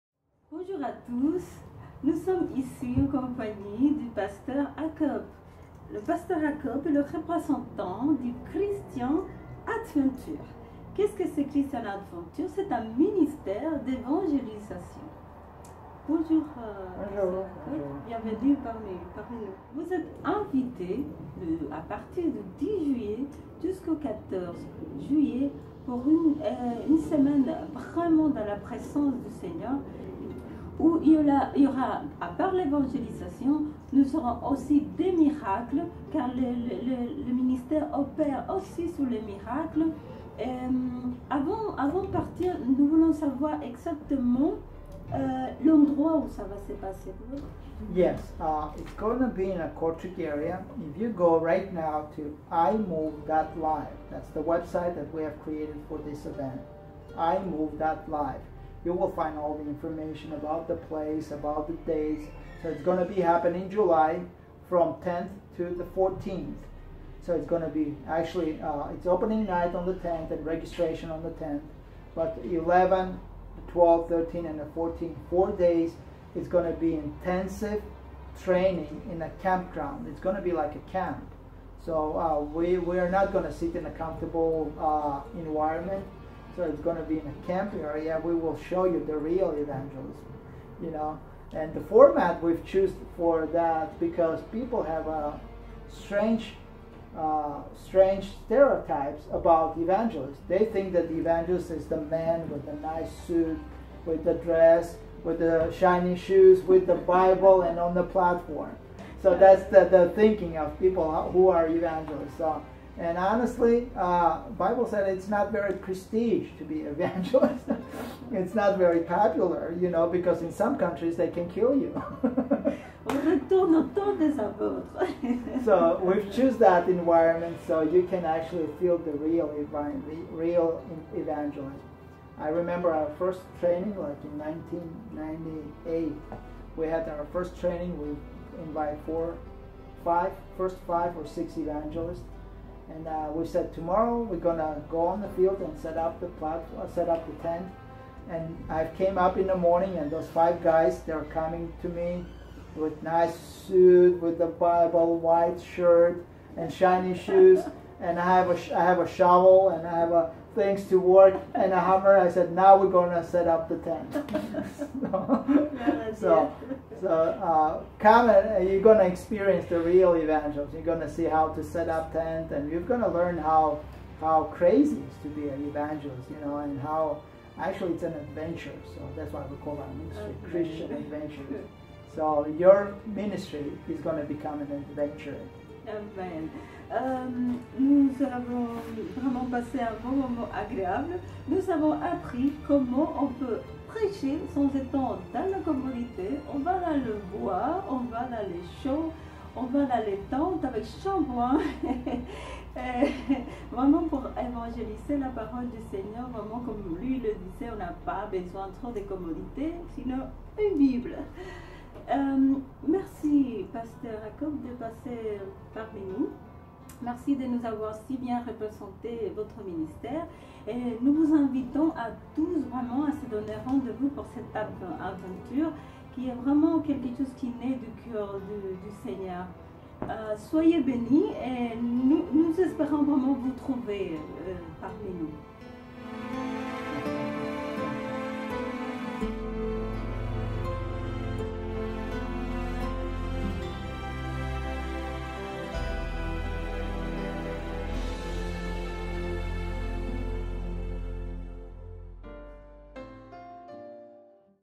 iMOVE - Interview avec le Pst. Akop (Réveilla)
Interview avec le Past. Akopà à propos de l'évènement ”iMOVE Belgium 2017“

Évènement:
Lieu :
Kapelhoekstraat 70
8511 Aalbeke, Belgique